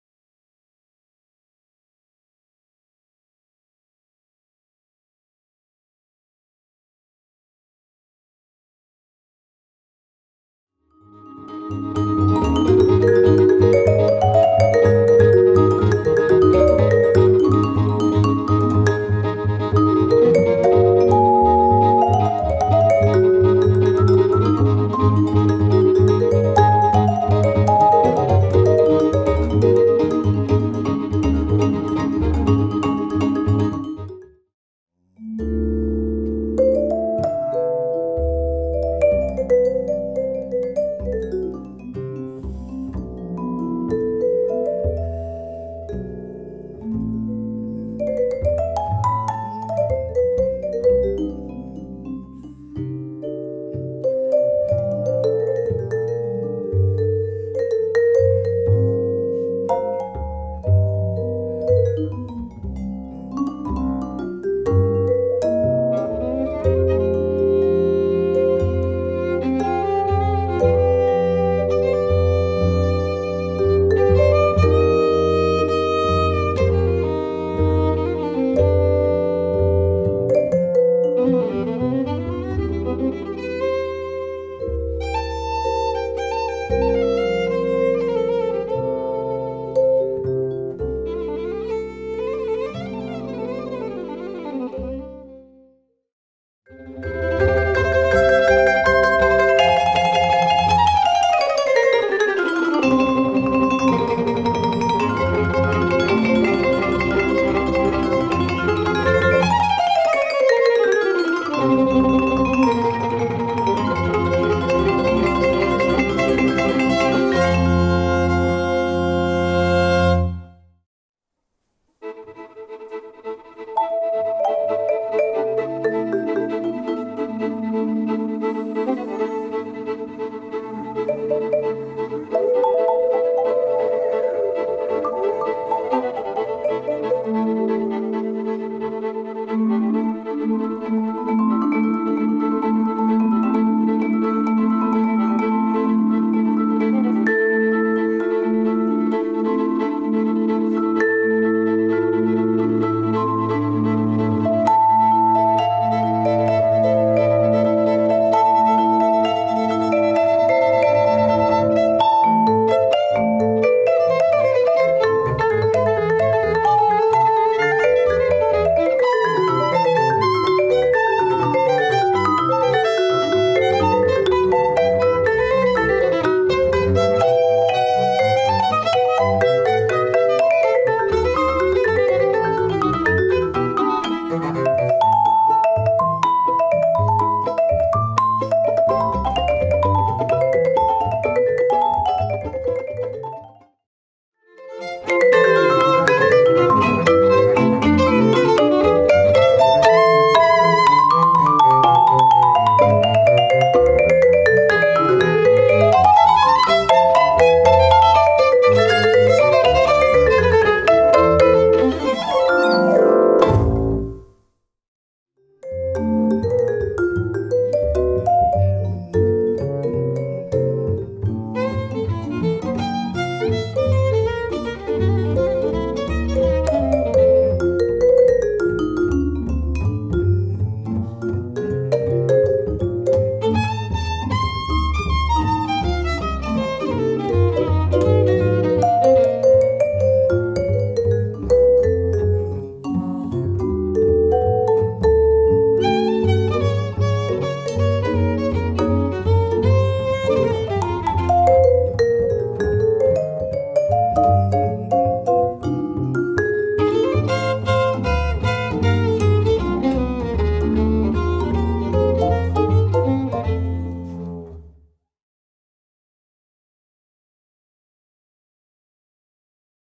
Thomas Dobler’s New Baroque – Live in Verzé - PercuSpace Resta-Jay
Enregistré au PercuSpace Resta-Jay le 1er février 2020

Adam Taubitz – violin
Miquel Angel Cordero – double bass
Thomas Dobler – vibes